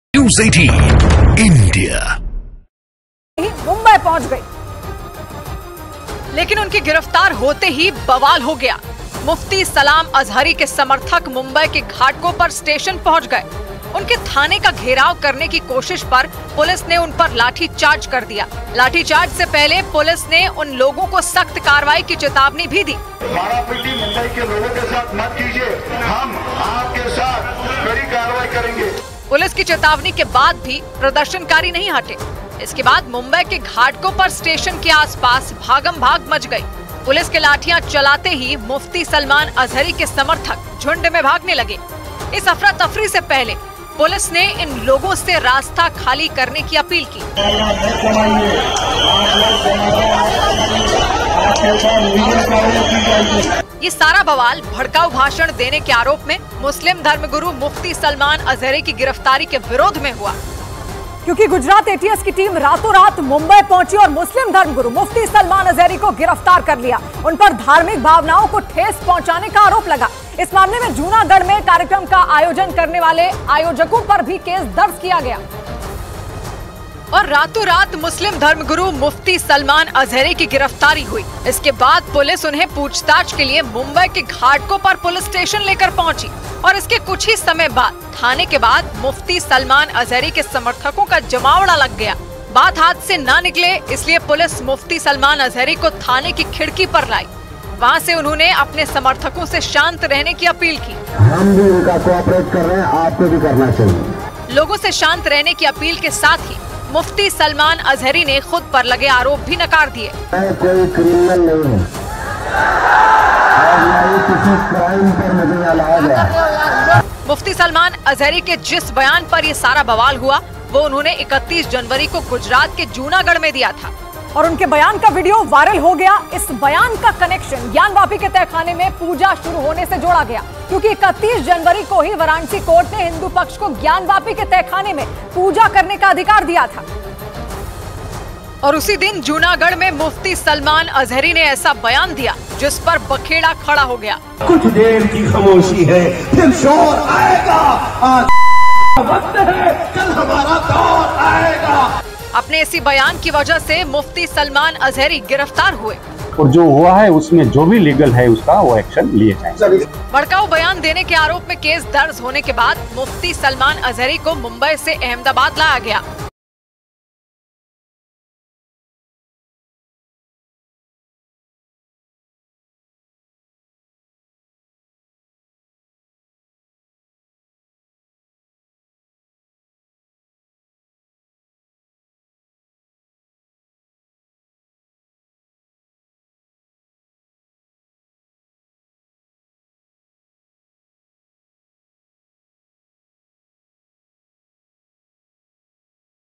Mufti Salman Azhari | रस्ते में ले जाते मुस्लिम धर्मगुरु पर घेर किया 'हमला' ? | Hate Speech |News18
Mufti Salman Azhari | भरस्ते में ले जाते मुस्लिम धर्मगुरु पर घेर किया 'हमला' ? | Hate Speech |News18